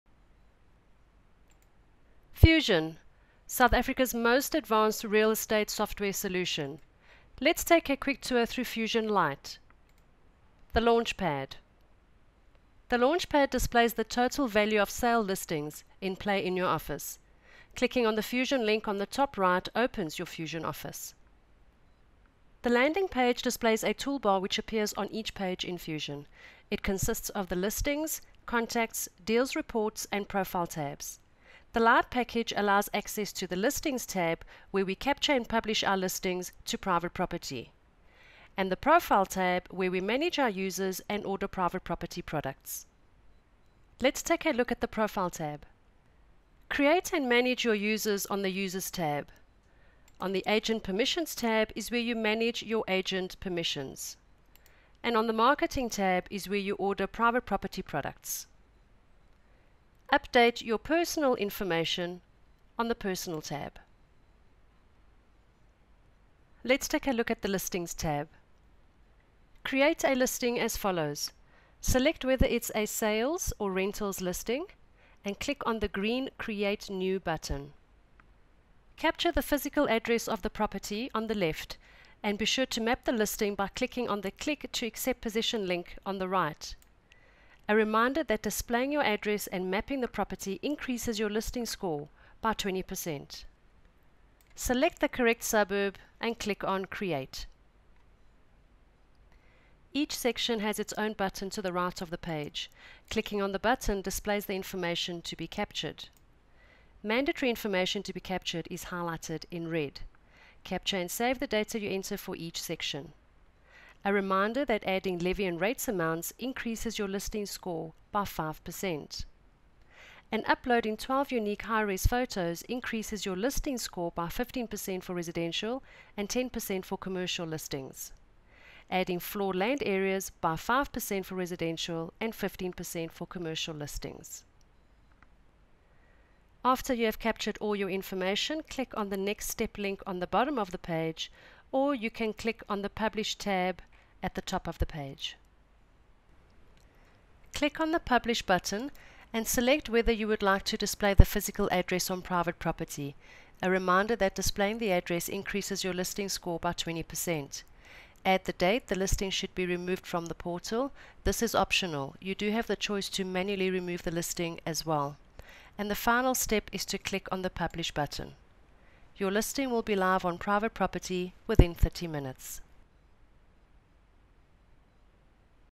Fusion: LITE Demo
This demonstration highlights Fusion Lite’s key features as well as how to capture and publish a listing